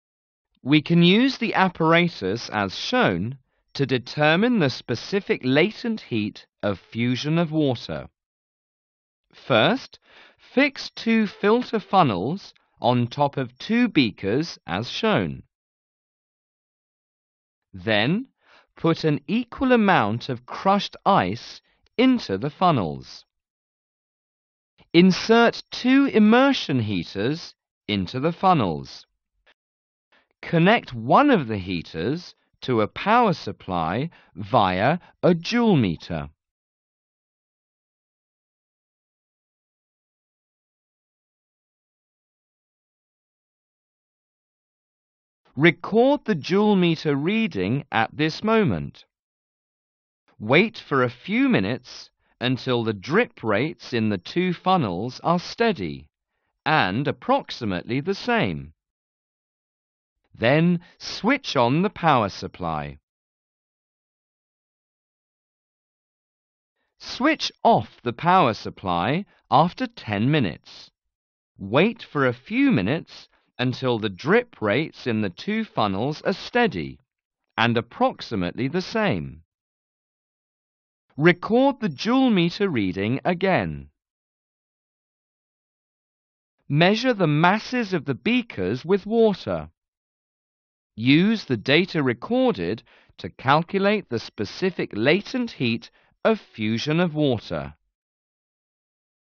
V03 e32b Ice specific latent heat of fusion II
00:00 set up
00:09 prepare two sets of apparatus
00:50 start heating when the drip rates are nearly the same
01:09 stop heating and wait the drip rates become nearly the same
01:24 measure the energy used and masses of beakers